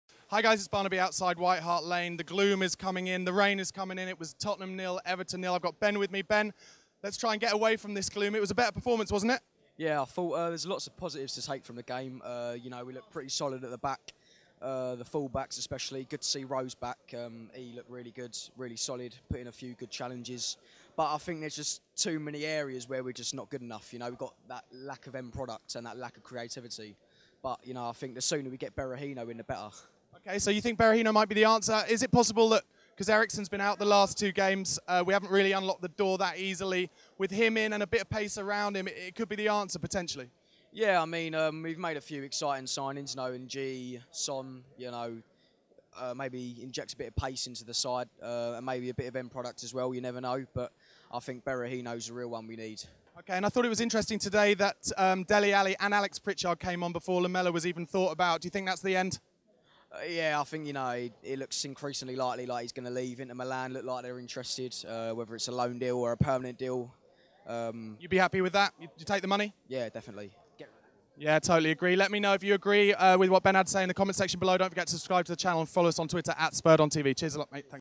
"We Were Solid At The Back!" | Spurs 0-0 Everton Fancam | Spurred On
This young fan thought we looked more solid at the back in the Spurs 0-0 Everton game.

Facebook.com/coysnews